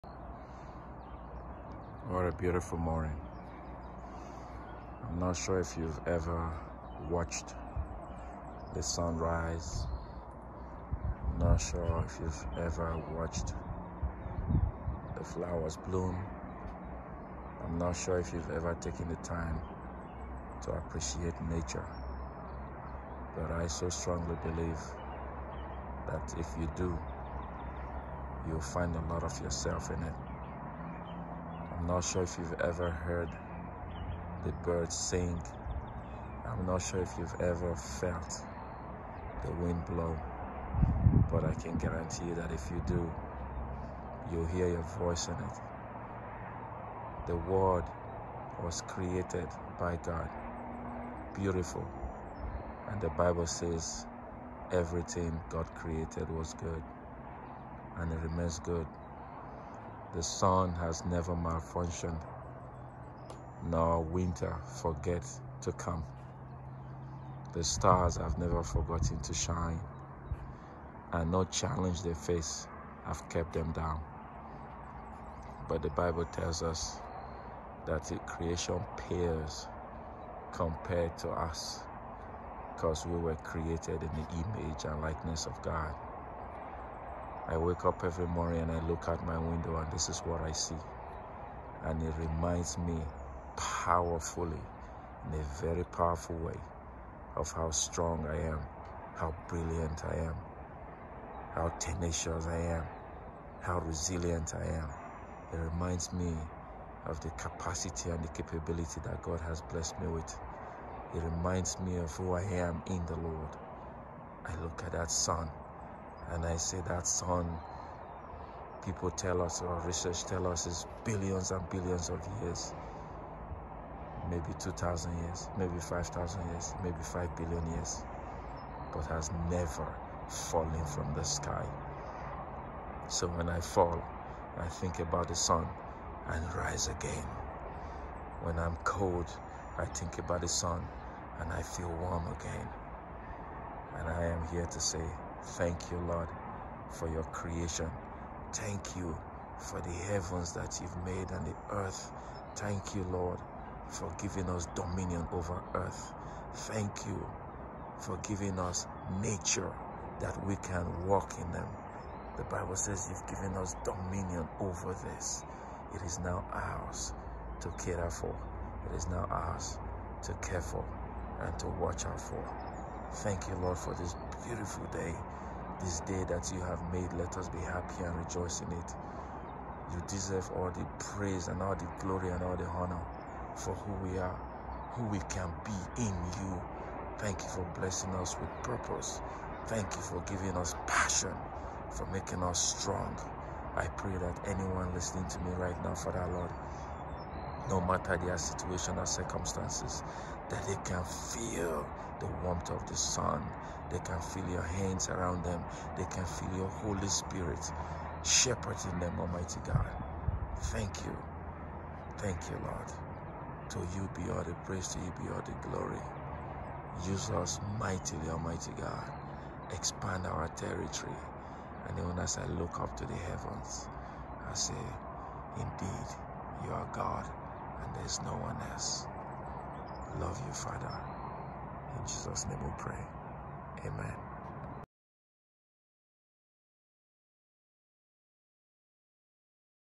The Greatness of Nature in YOU
I woke up this morning by the grace of God and looked out my window and saw the sun rise, heard the birds sing and the flowers bloom.

I couldn’t help but be reminded so powerfully that nature's majesty stands as a timeless testament to the beauty and power that resides within each of us.

From the towering mountains to the gentle whispers of a flowing stream, nature's grandeur whispers a profound truth: that within us lies a strength and greatness waiting to be unleashed.

Just as the mighty oak grows from a small seed, so too can we nurture our inner potential to achieve remarkable feats.

Let the awe-inspiring wonders of nature serve as a constant reminder of the boundless possibilities that await when we embrace our own inherent power.

By aligning ourselves with the rhythms of the natural world, we unlock a source of endless inspiration and empowerment, propelling us to reach greater heights and forge our own paths of greatness.